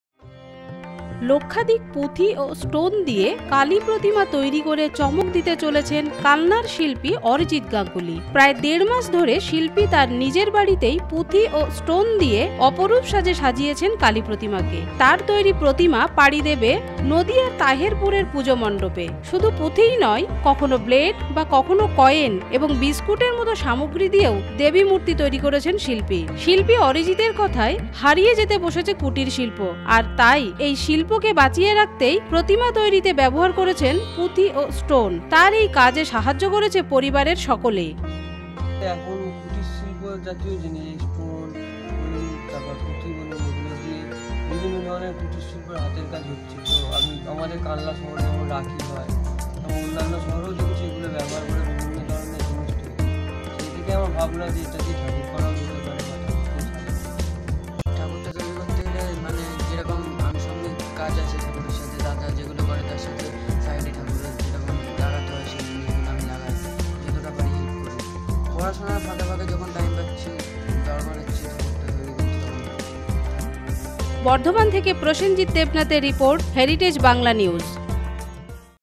লক্ষাধিক পুথি ও স্টোন দিয়ে কালী প্রতিমা গড়ে নজর কাড়লেন, পূর্ব বর্ধমানের অরিজিৎ গাঙ্গুলী
লক্ষাধিক পুথি ও স্টোন দিয়ে কালী প্রতিমা গড়ে নজর কাড়লেন, পূর্ব বর্ধমানের কালনার শিল্পী অরিজিৎ গাঙ্গুলী